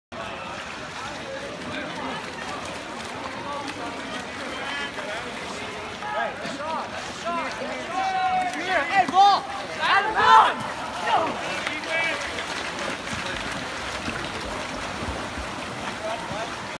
99/15/17 - Nick C. H2O Polo play action
Modesto F/S tournament day 2 game 2.
7:6win!